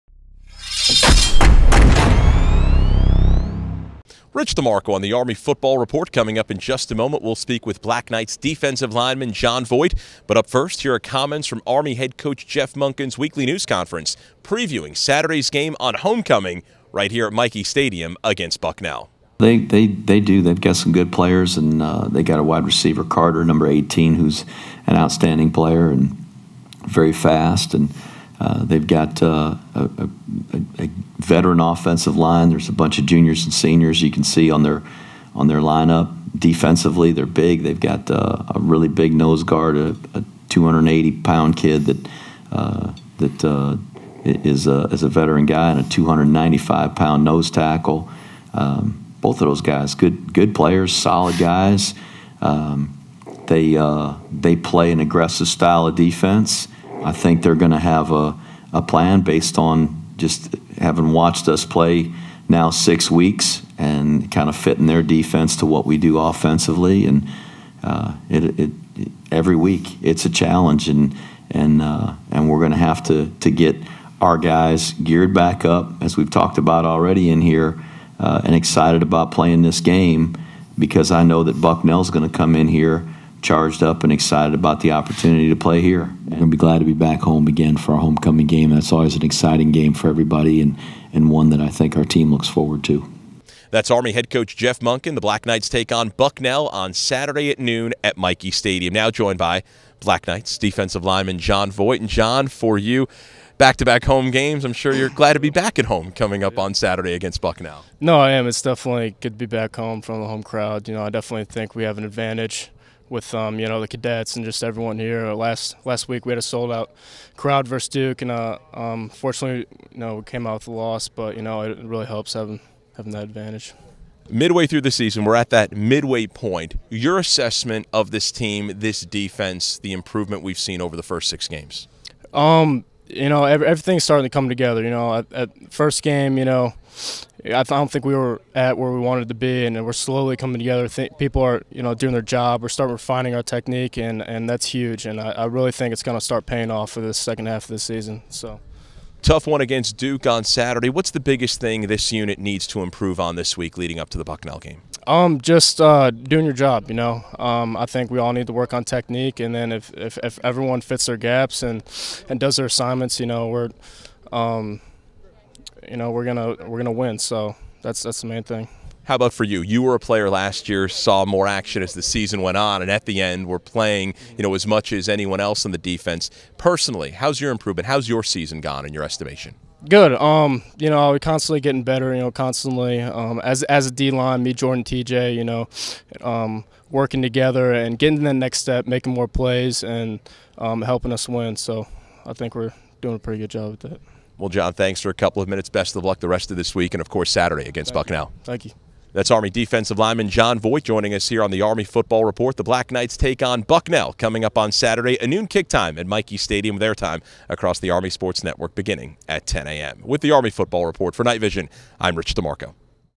Army West Point Football Report: 10-13-15
Rich DeMarco talks to John Voit as we hit the half way point on the season and Jeff Monken previews the matchup against Bucknell.